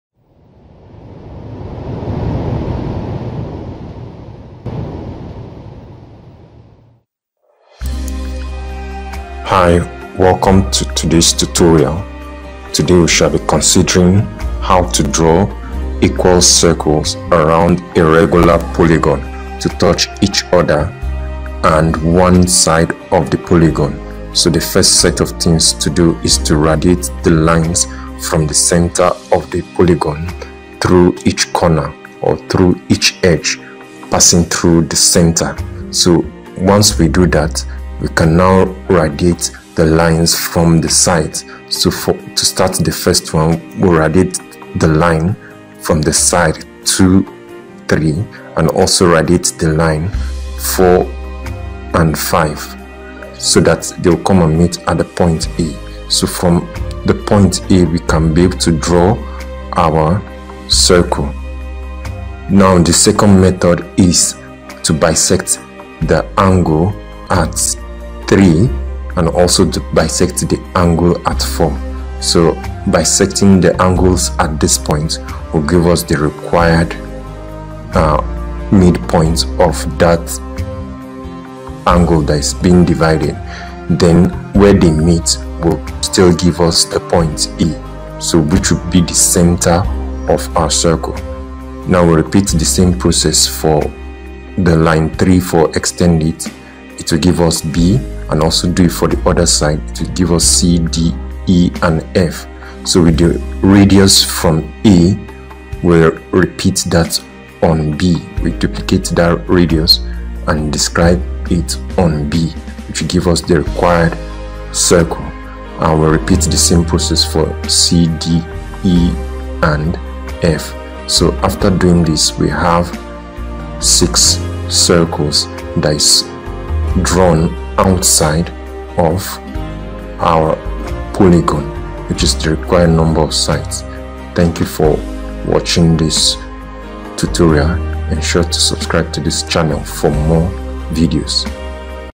Engineering Drawing || draw equal circles outside a regular polygon to touch each other
The construction of equal circle outside a regular polygon such as hexagon